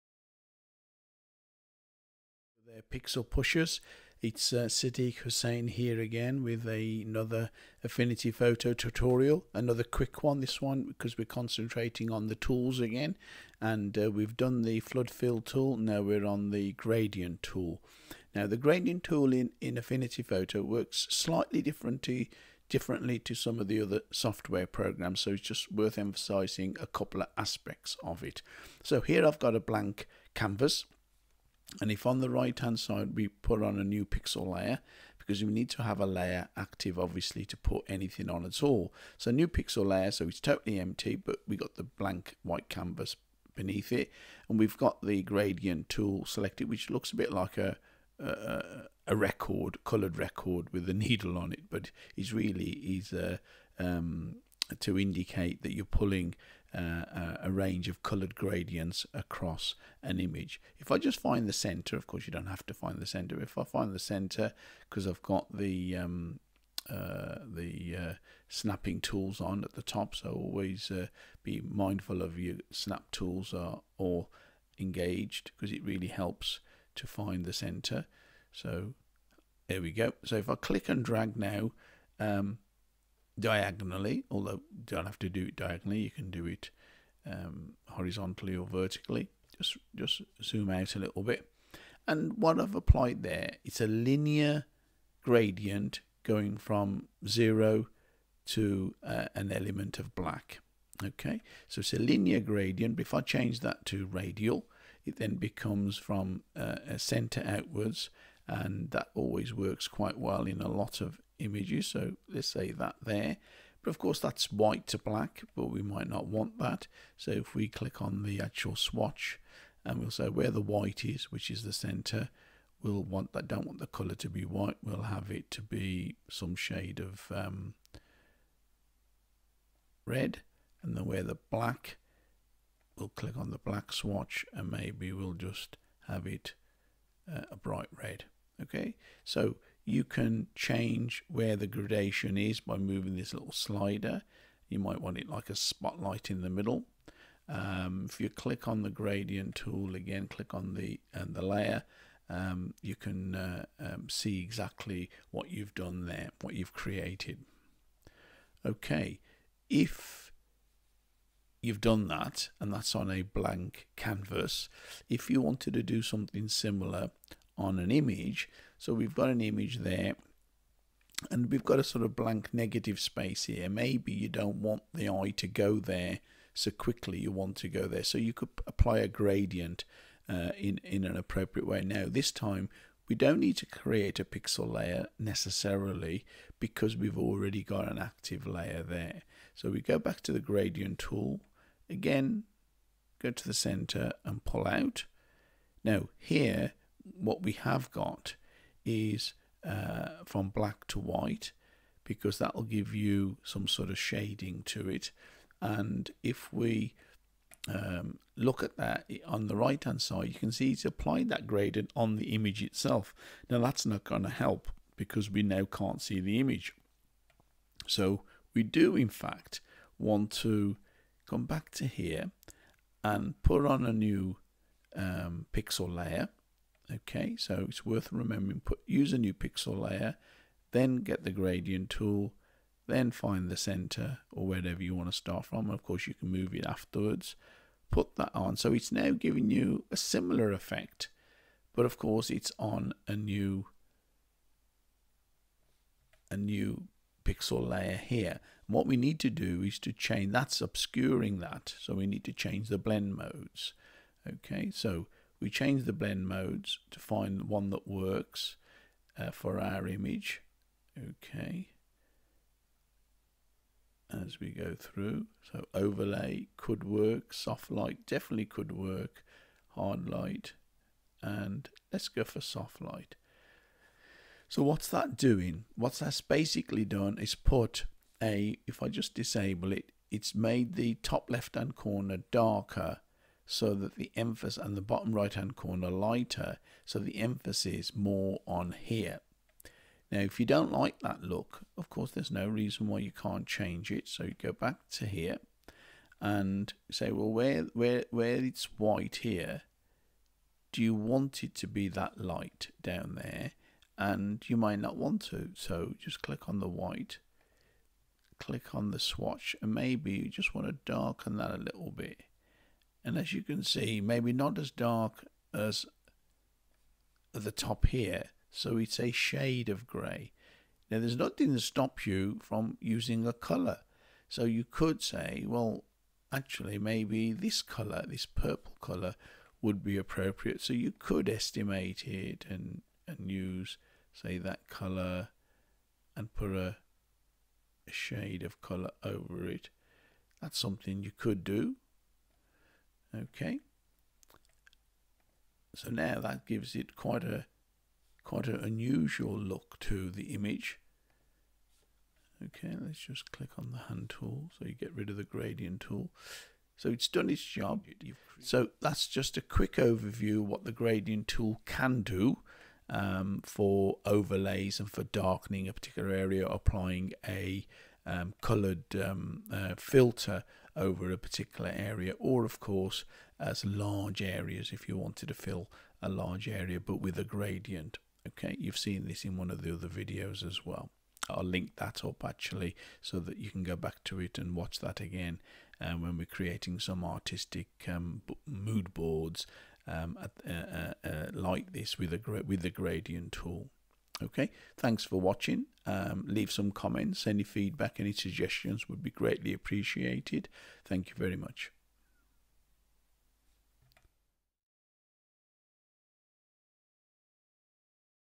Affinity Photo - Gradient Tool & Tips
As we go through the Affinity Photo Tools, we have got to the very useful and versatile Gradient Tool. Use this to create different types of gradients on blank canvasses, artwork or mood boards to add extra interest. Or it can used as a graduated filter on steroids. Check it out.